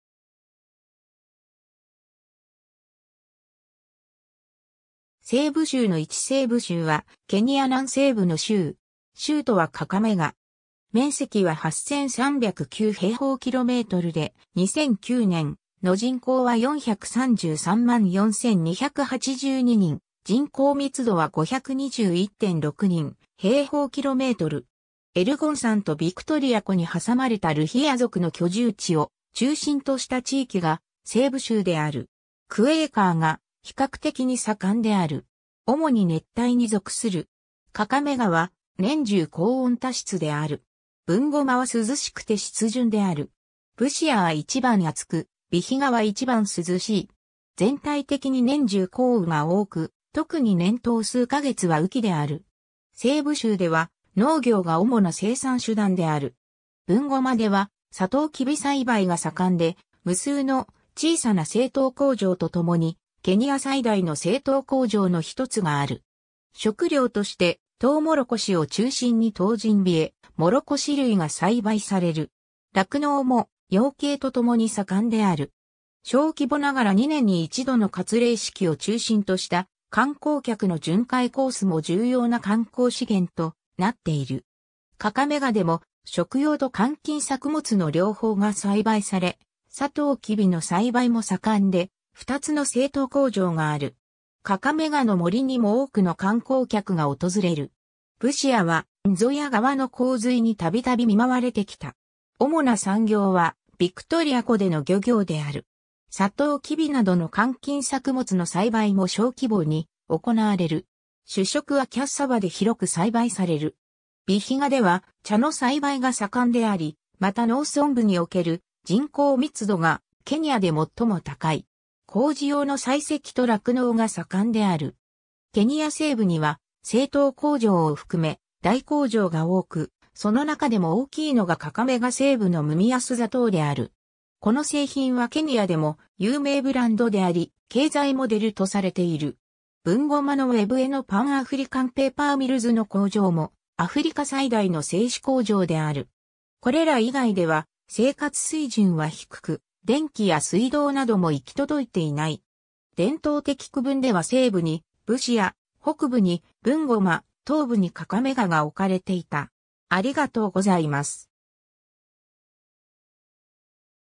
西部州 (ケニア)
西部州の位置 西部州(せいぶしゅう、スワヒリ語：Mkoa wa Magharibi)はケニア南西部の州。
 州都はカカメガ。
 面積は8309km2で、2009年の人口は433万4282人、人口密度は521.6人/km2。
 エルゴン山とヴィクトリア湖に挟まれたルヒヤ族の居住地を中心とした地域が西部州である。
 クエーカーが比較的に盛んである。
 主に熱帯に属する。
 カカメガ (カウンティ)は年中高温多湿である。
 ブンゴマ (カウンティ)は涼しくて湿潤である。
 ブシア (カウンティ)は一番暑く、ヴィヒガ (カウンティ)は一番涼しい。
 全体的に年中降雨が多く、特に年頭数ヶ月は雨期である。
 西部州では農業が主な生産手段である。
 ブンゴマ (カウンティ)ではサトウキビ栽培が盛んで、無数の小さな製糖工場と共にケニア最大の製糖工場の一つがある。
 食料としてトウモロコシを中心にトウジンビエ、モロコシ類が栽培される。
 酪農も養鶏と共に盛んである。
 小規模ながら二年に一度の割礼式を中心とした観光客の巡回コースも重要な観光資源となっている。
 カカメガ (カウンティ)でも食用と換金作物の両方が栽培され、サトウキビの栽培も盛んで、2つの製糖工場がある。
 カカメガの森にも多くの観光客が訪れる。
 ブシア (カウンティ)はンゾイア川の洪水に度々見舞われてきた。
 主な産業はビクトリア湖での漁業である。
 サトウキビなどの換金作物の栽培も小規模に行われる。
 主食はキャッサバで広く栽培される。
 ヴィヒガ (カウンティ)では茶の栽培が盛んであり、また農村部における人口密度がケニアで最も高い。
 工事用の採石と酪農が盛んである。
 ケニア西部には製糖工場を含め大工場が多く、その中でも大きいのがカカメガ (カウンティ)西部のムミアス砂糖である。
 この製品はケニアでも有名ブランドであり、経済モデルとされている。
 ブンゴマ (カウンティ)のウェブエのパンアフリカンペーパーミルズの工場もアフリカ最大の製紙工場である。
 これら以外では生活水準は低く、電気や水道なども行き届いていない。
 伝統的区分では西部にブシア、北部にブンゴマ、東部にカカメガが置かれていた。